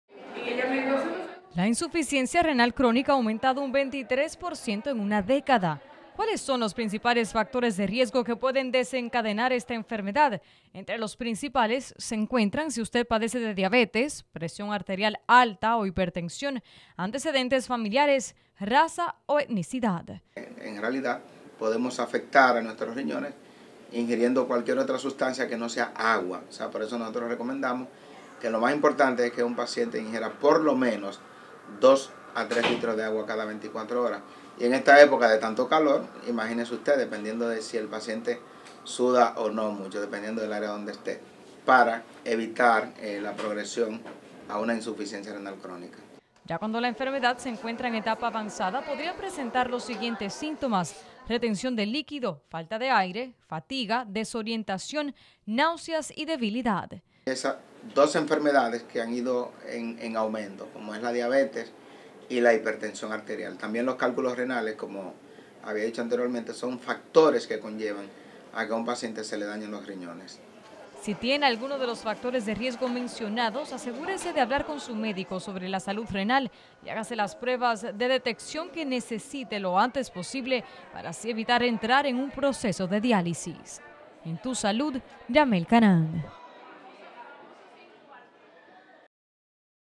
La insuficiencia renal ataca de manera repentina al 13% de los dominicanos
Es una enfermedad silenciosa, crónica y degenerativa que suele no presentar síntomas y atacar de forma repentina.